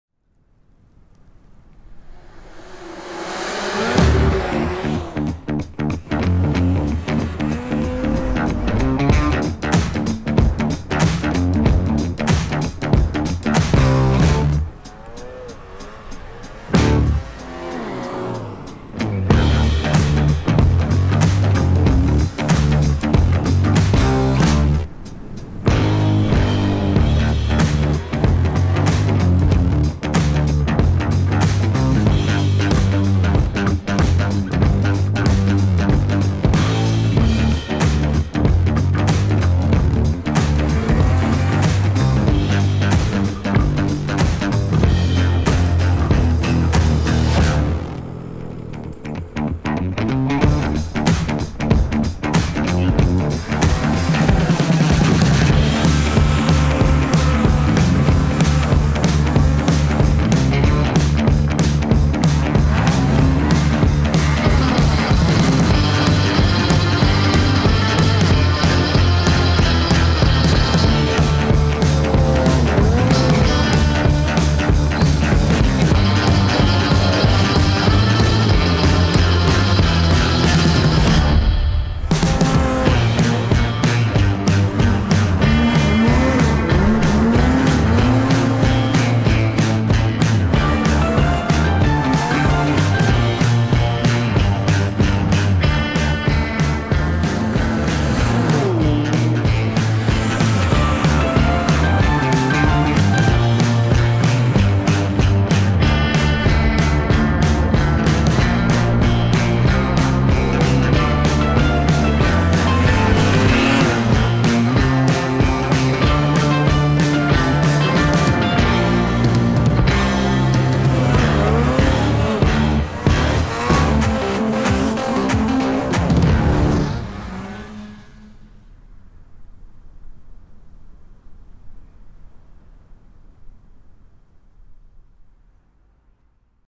Lynx Xterrain: Pro, RE & RE Turbo | Lynx Lineup 2020
The all new Lynx Xterrain by Lynx Snowmobiles. Available as Pro, RE and RE Turbo models.